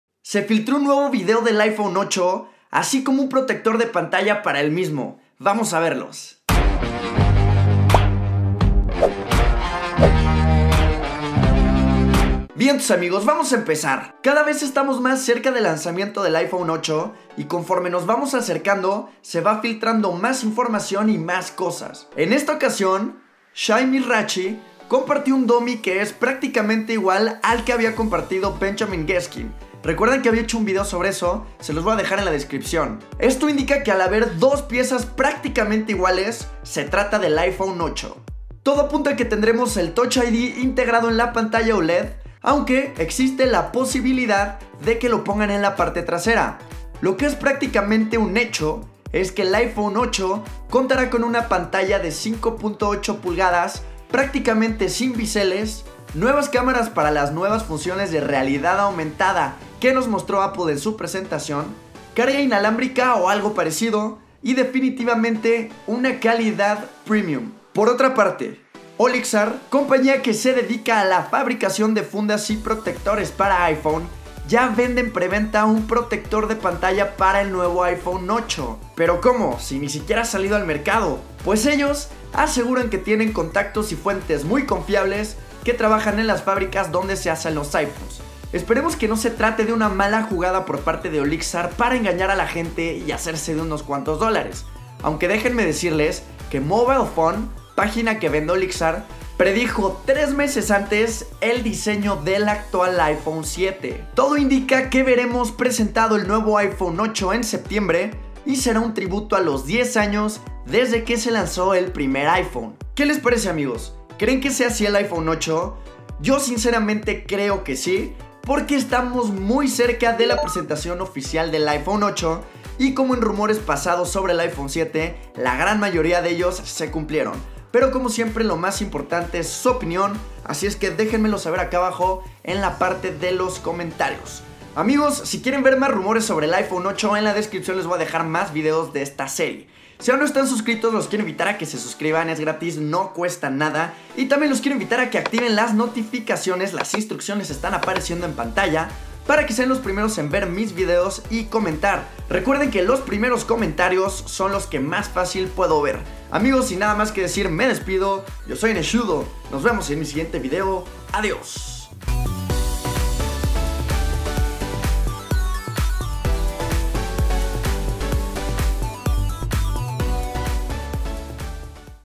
iPHONE 8 EN VIDEO - FILTRADO!
Que onda que tal amigos,

En esta ocasión les traigo este video sobre los videos más nuevos que se filtraron del iPhone 8.

GRACIAS!